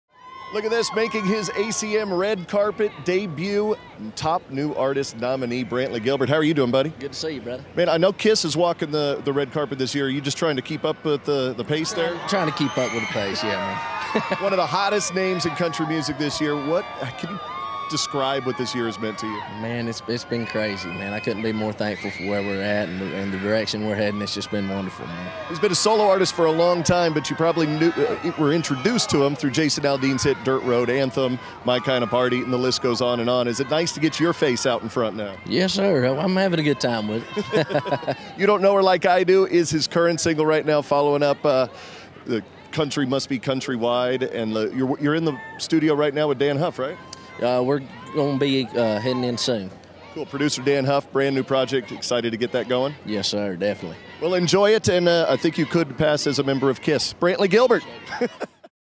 Brantley Gilbert Red Carpet Interview ACM Awards 2012
Brantley Gilbert, who is nominated for Best New Artist, says that this past year has been crazy and that it's made him want to work even harder.